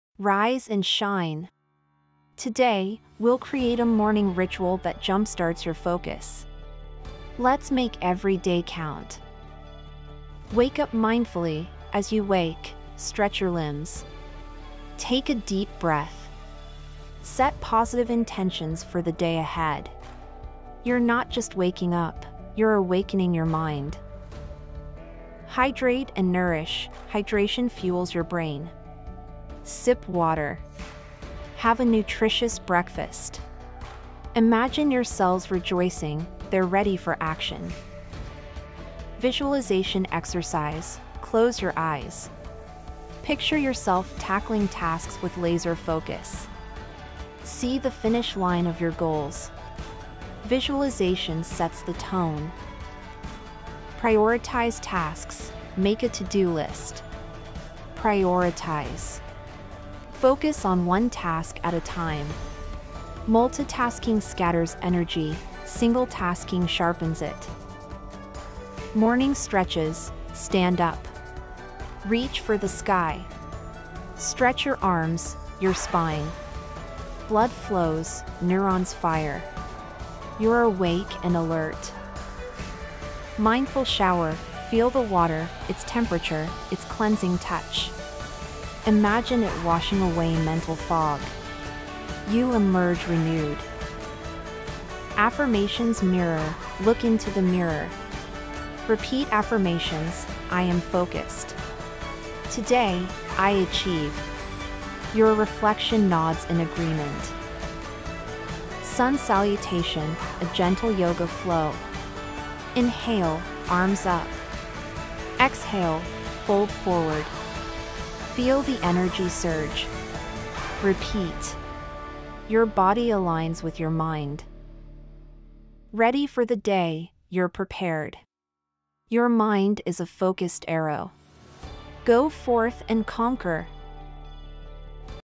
Morning Ritual: Boosting Concentration
Rise with intention. This video unveils a morning ritual—a sacred dance that sets the rhythm for your day. Imagine waking mindfully—stretching your limbs, feeling the stretch awaken your muscles.

Take a deep breath—the first sip of life—setting positive intentions. Hydrate; nourish your brain with a nutritious breakfast. Picture your cells rejoicing—they’re ready for action. Visualization becomes your morning muse—see yourself tackling tasks with laser focus, crossing finish lines, achieving milestones.

Prioritize—make a to-do list, but don’t stop there. Single-task your way to productivity; scatter your energy no more. Incorporate morning stretches—your body, a canvas for clarity. Mindful showers—feel the water cleanse not just your skin but also mental fog.

Affirmations mirror—look into your eyes and repeat: “I am focused,” “Today, I achieve.” Sun salutations—inhale, arms up; exhale, fold forward. Blood rushes, neurons fire. You’re awake and alert.

Ready for the day, you step into the world—a focused arrow, a purposeful breath. The morning ritual isn’t just a routine; it’s your secret weapon against distraction.